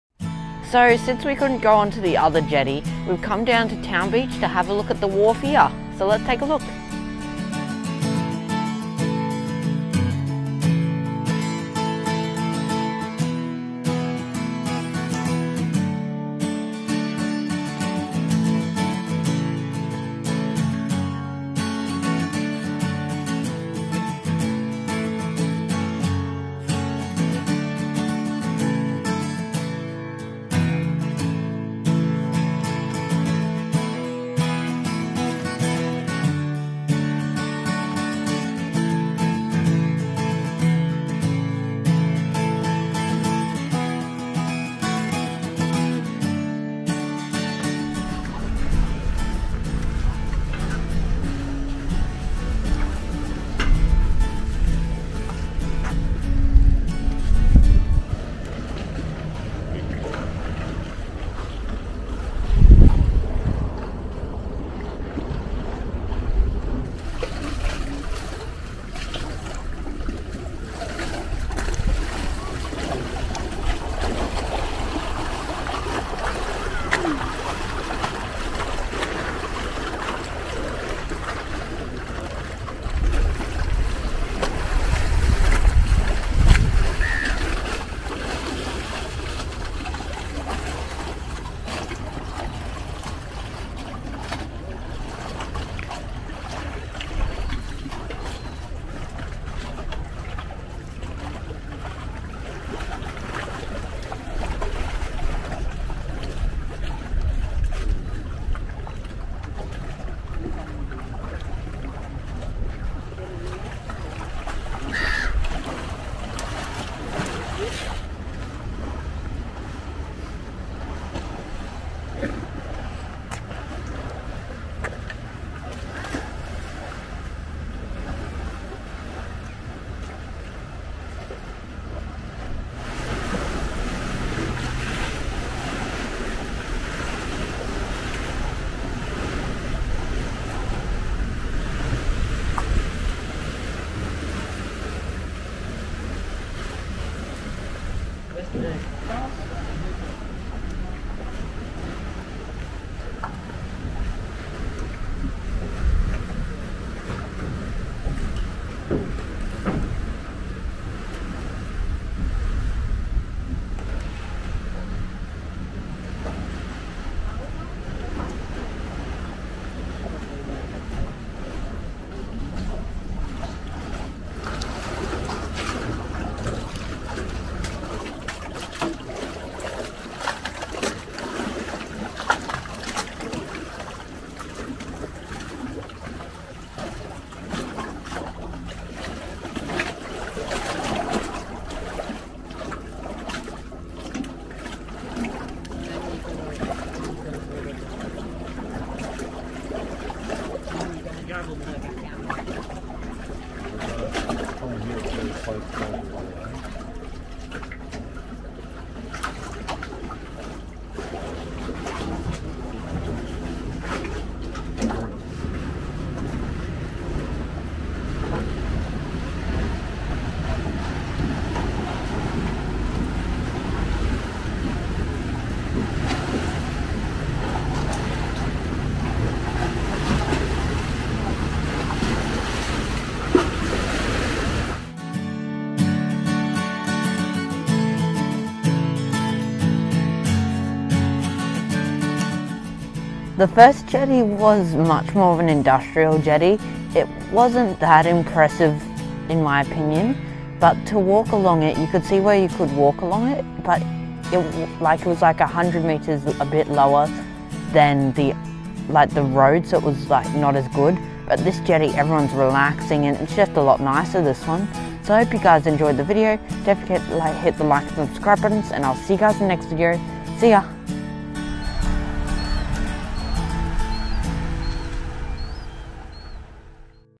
WA Holiday 2022 | Day #20 Video #3 | Town Beach Jetty, Broome
8 July 2022

This video is of our visit to the Croc Town Beach Jetty. It was awesome!